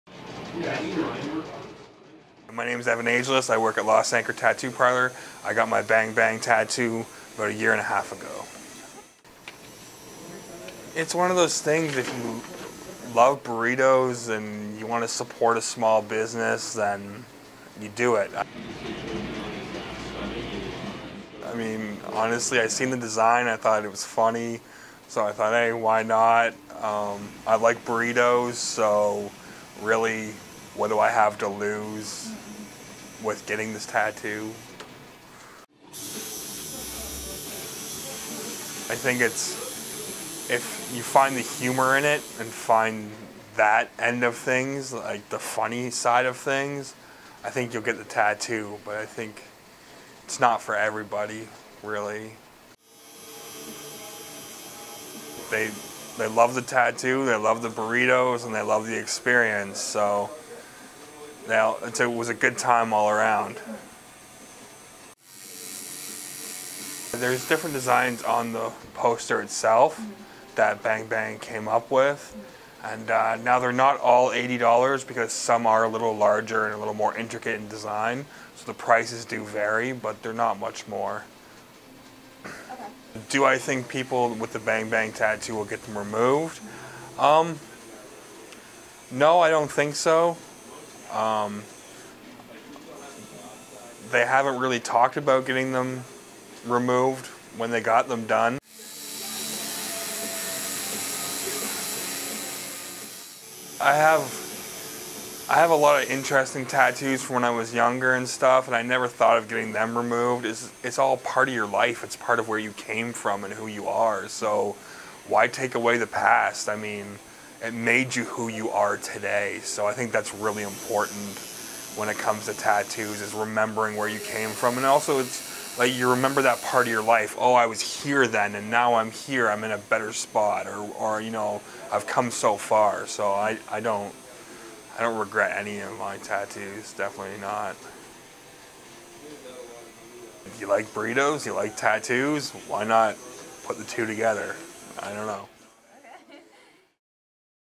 Evan Ageless gets Bang Bang Tattoo
Bang Bang Burrito on 1812 Simcoe Street North has garnered attention for their sales gimmick. If you get a tattoo of BangBang, you get a year of free burritos. Sounds crazy right? But people are actually doing it.